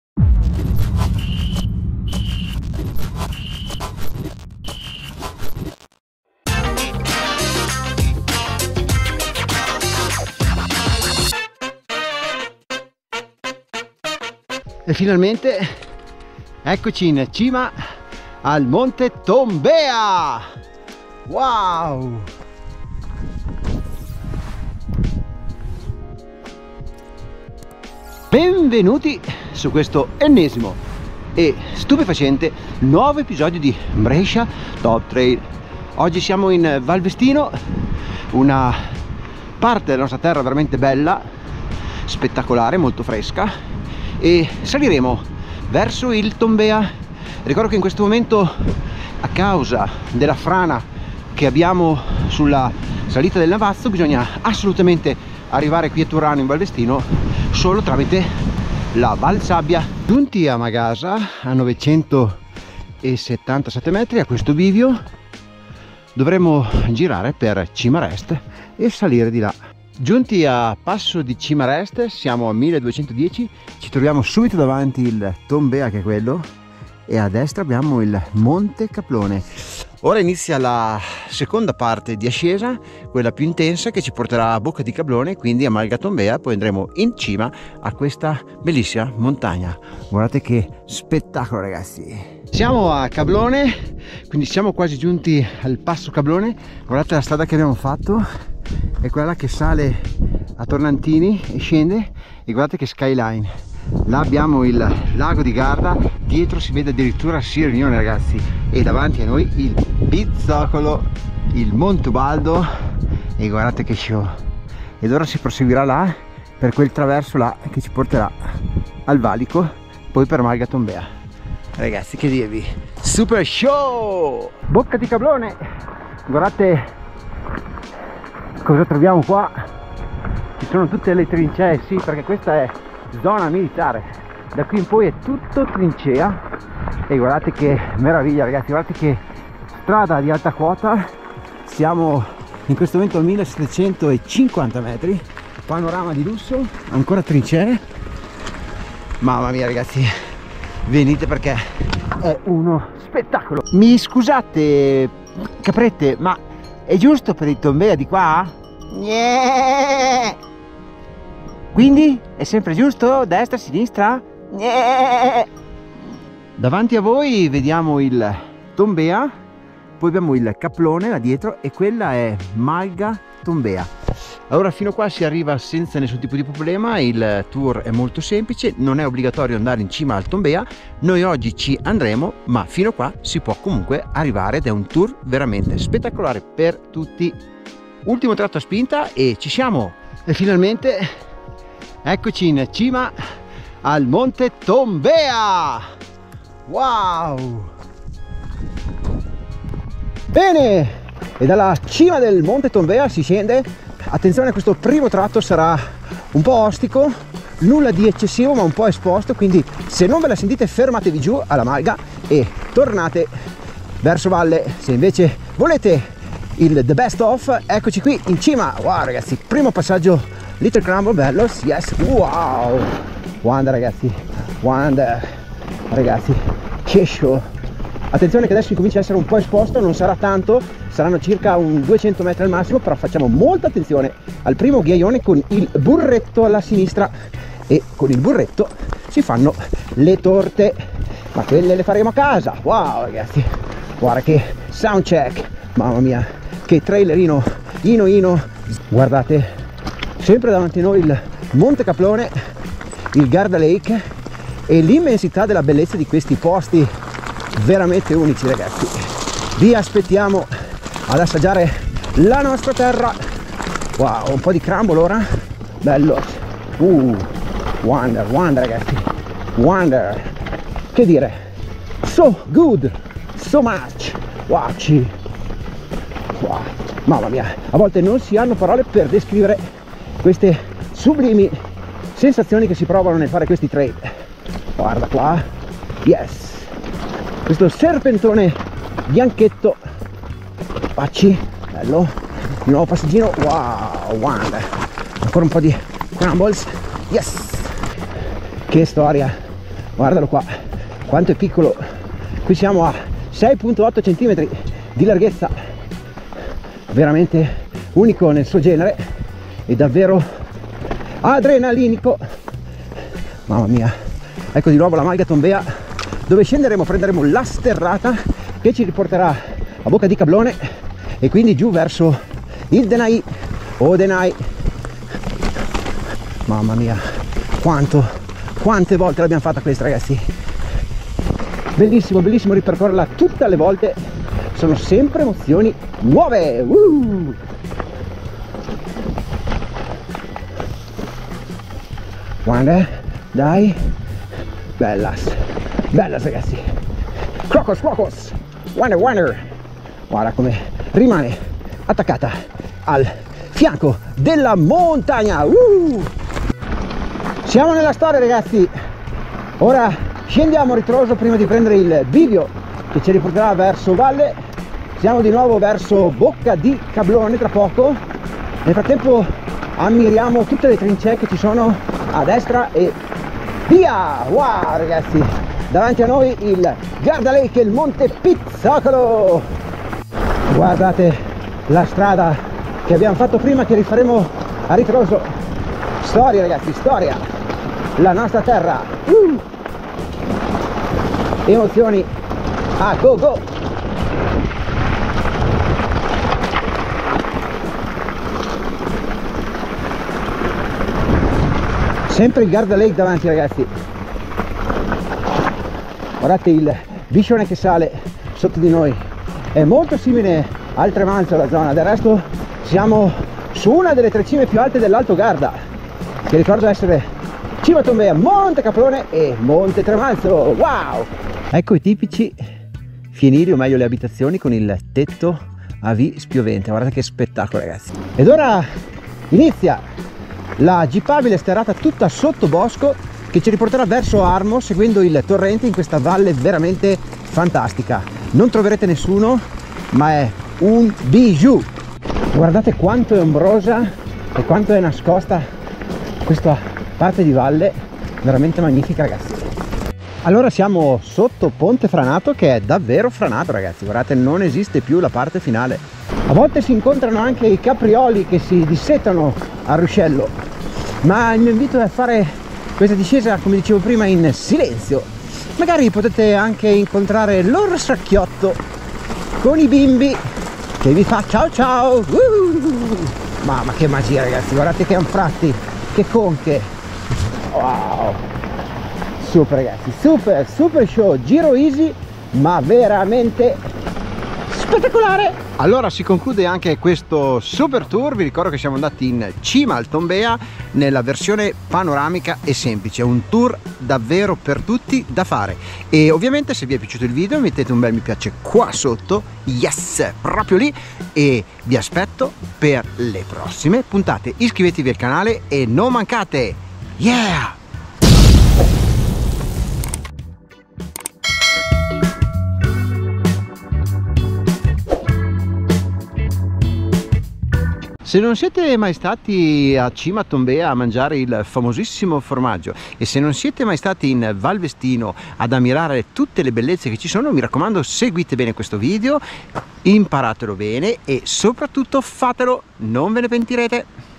VADO IN PARADISO IN TOMBEA. Che tour spettacolare....
Ho deciso di portarvi con me in cima al Monte Tombea con un tour semplice e panoramico fatto di mille emozioni sublimi. Ovviamente in sella alla Moustache Game 3 by @EBIKESTOREBRESCIA
Vi invitiamo a seguirci su tutti i canali Social.